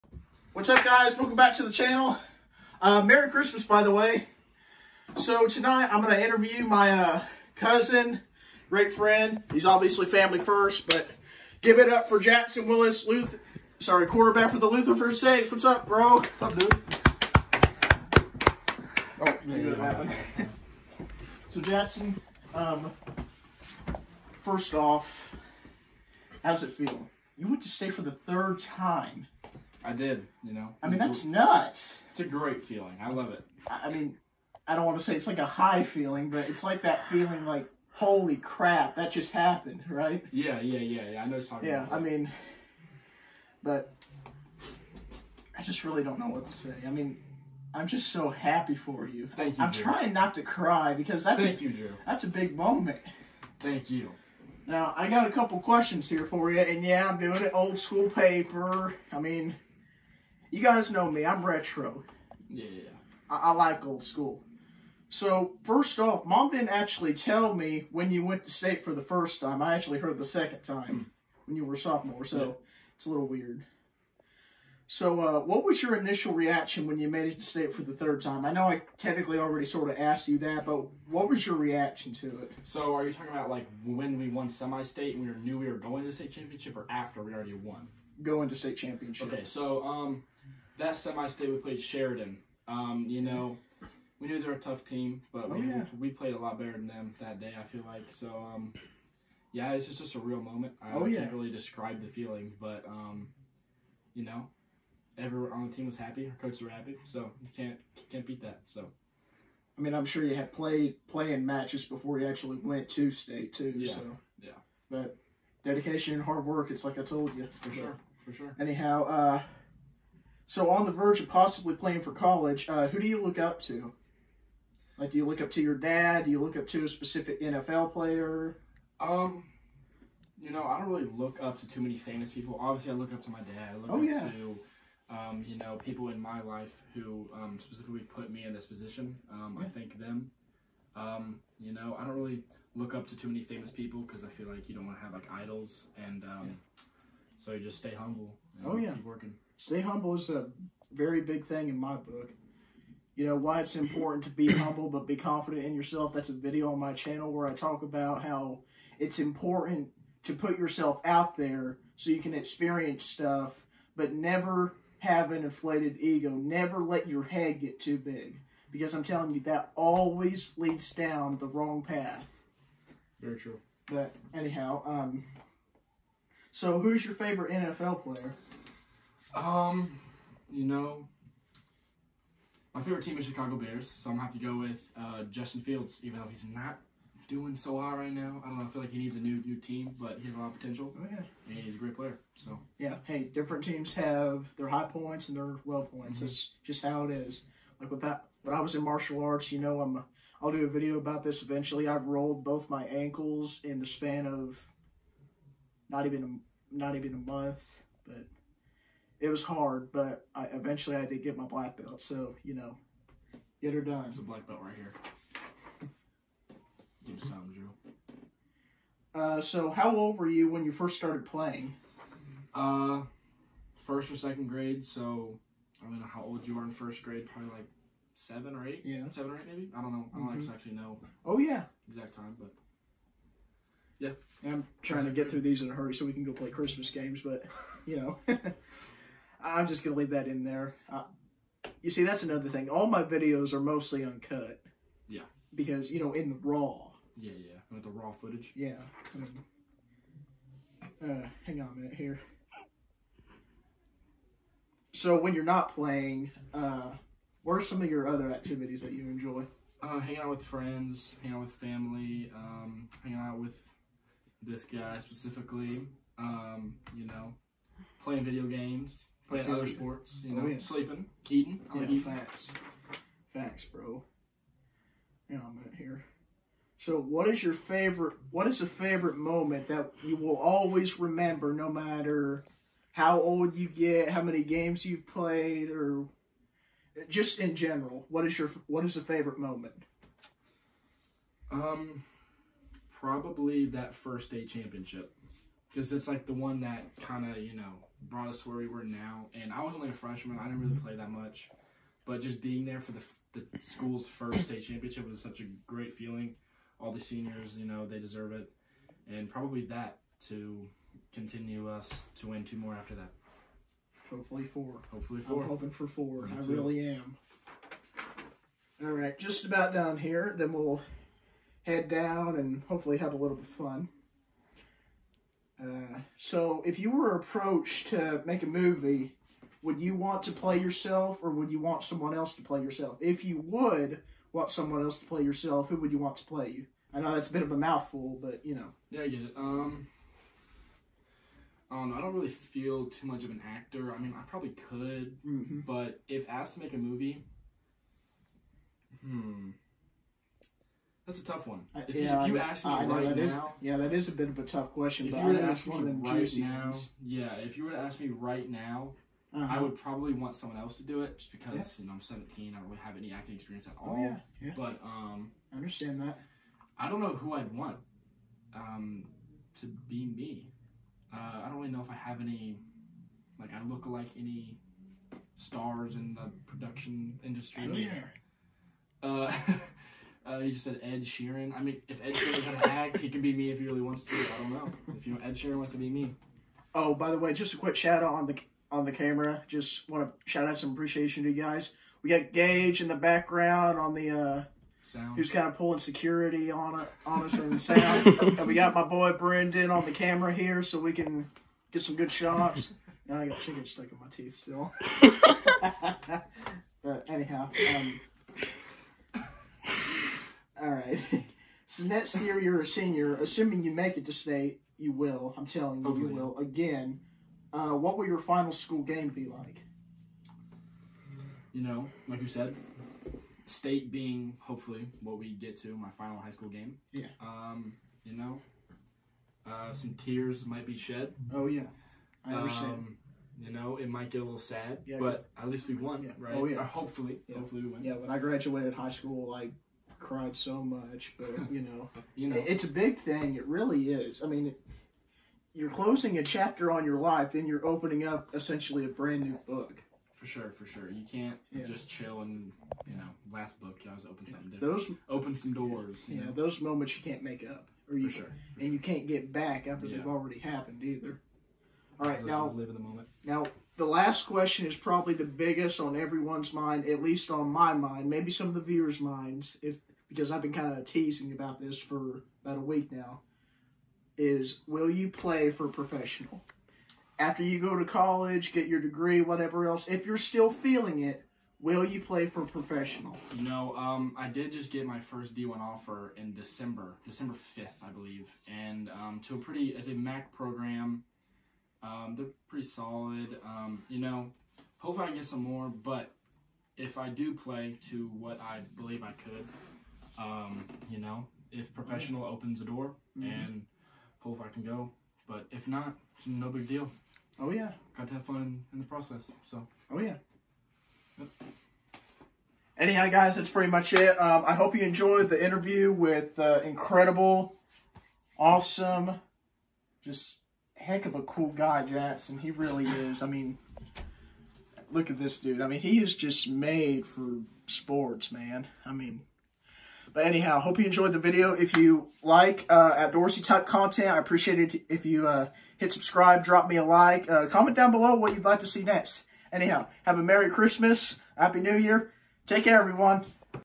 Jackson Willis QB for lutheren saints interview.
Jackson Willis Lutheran School QB
3 State wins in a row

    Also please checkout schoolcraft Family Farmstead! Link to his channel is bellow.